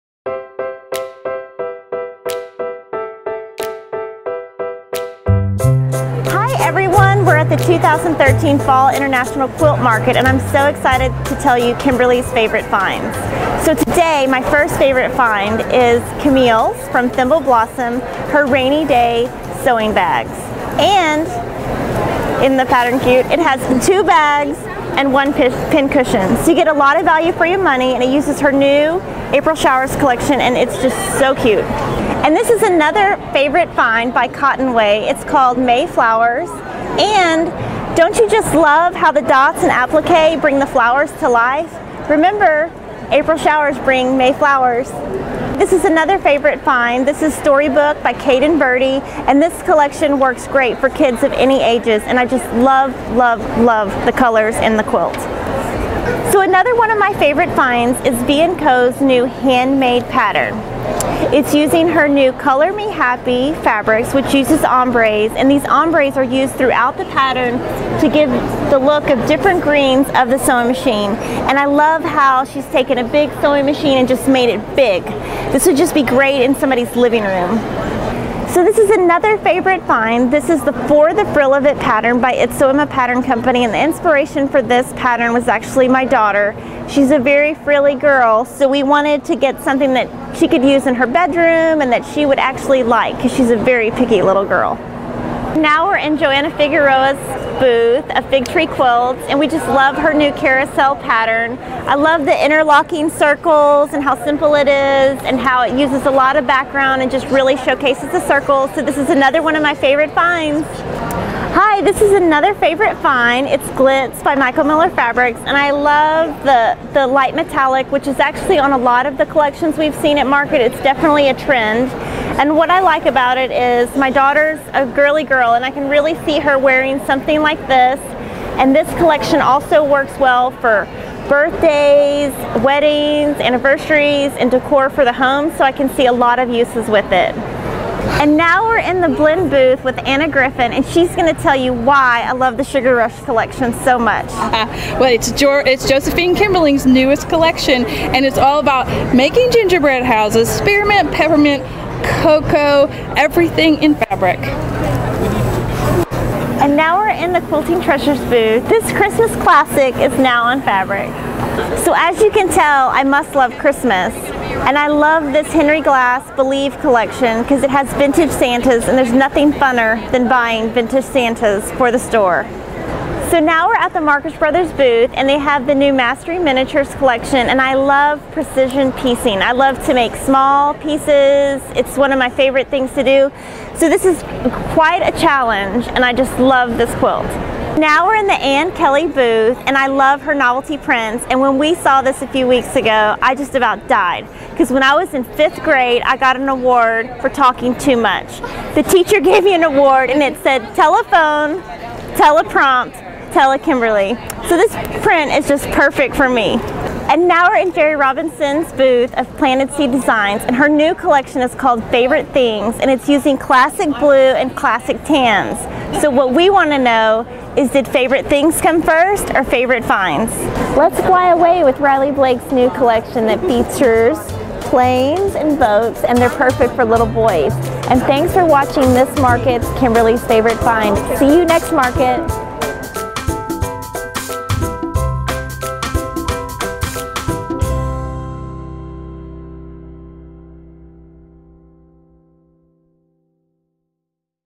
Kimberly's Favorite Finds at the Fall International Quilt Market 2013
Kimberly shares her favorite finds from Fall Quilt Market 2013 in Houston! Let's get excited about all these new and upcoming collections, patterns and Blocks of the Month!

Featuring:
Rainy Day Sewing Bags. Thimble Blossoms
Mayflowers. Cotton Way
Storybook. Kate & Birdie Paper Co.
Handmade. V & Co.
For the Frill of It. It's Sew Emma
Carousel. Fig Tree Quilts
Glitz. Michael Miller Fabrics
Sugar Rush .Blend Fabrics
Rudolph. .Quilting Treasures
Believe. Henry Glass
Mastering Miniatures Block of the Month. Homestead Hearth
This N That. Ann Kelle
Favorite Things. Gerri Robinson
Fly Aweigh. Riley Blake